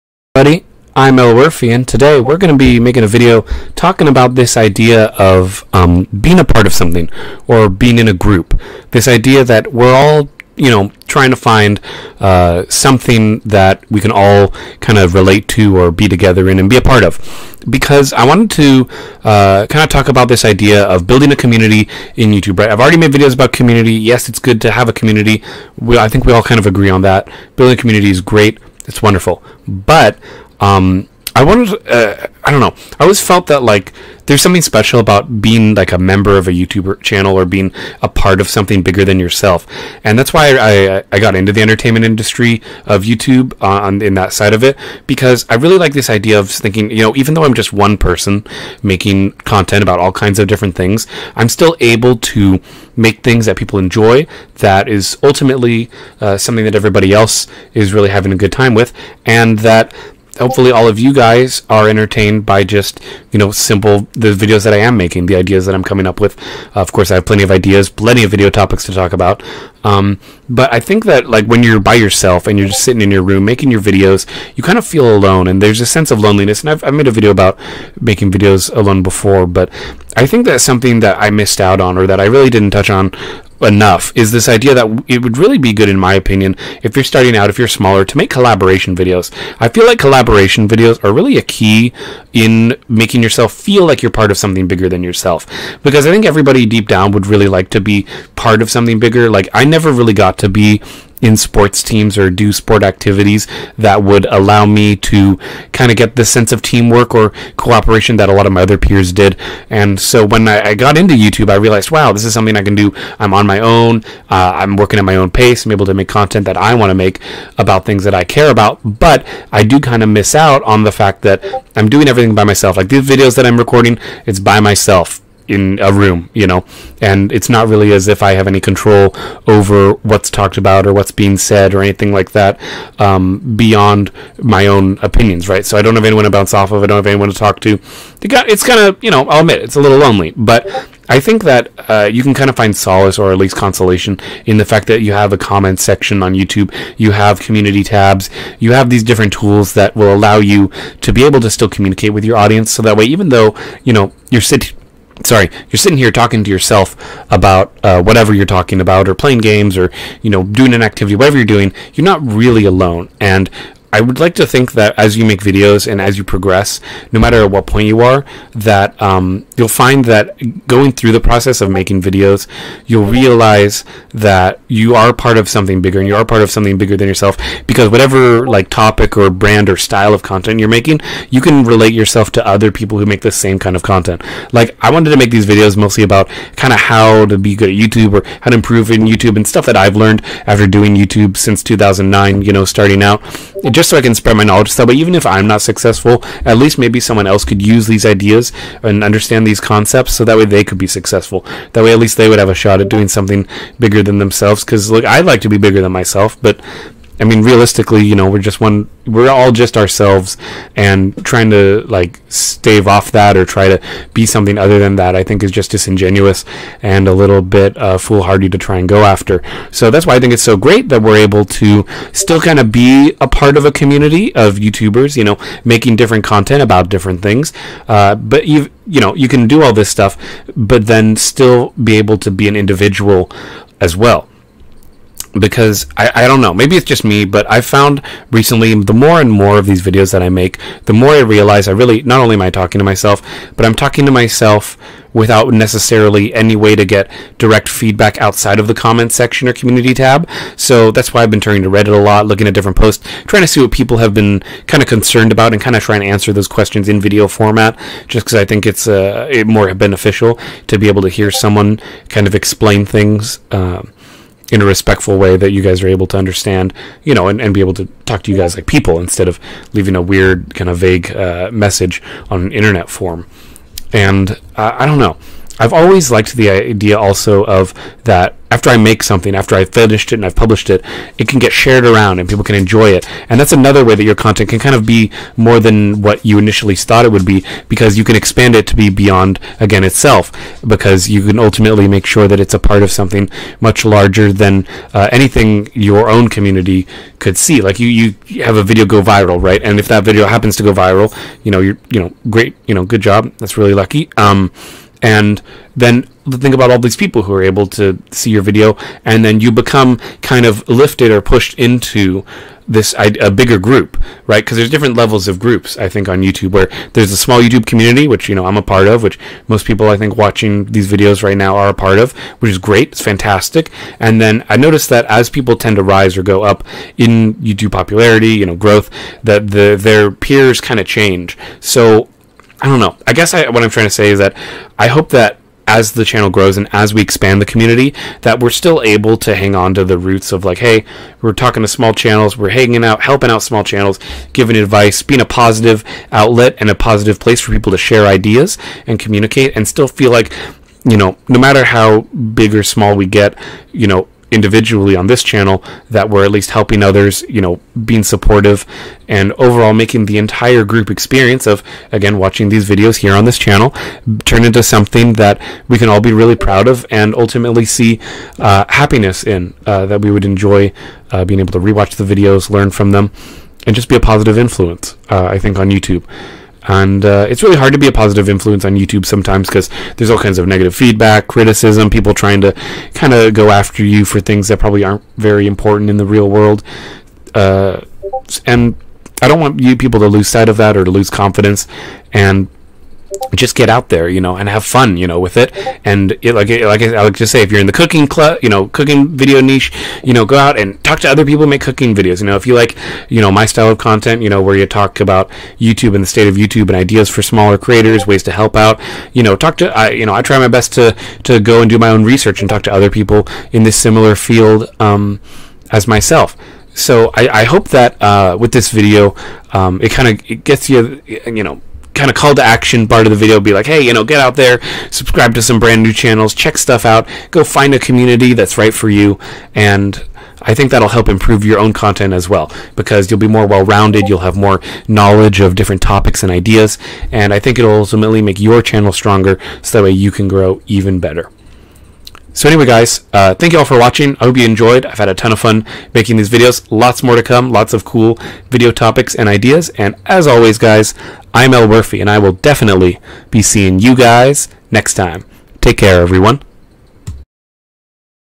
Being a Part of Something
Hey everyone! I'm happy to talk about this feeling I've been having about the way I think about my place in YouTube. I know that I've always wanted to do something bigger than myself. And do be a member of something truly special. I think I finally understand my role on this platform, and I'm proud.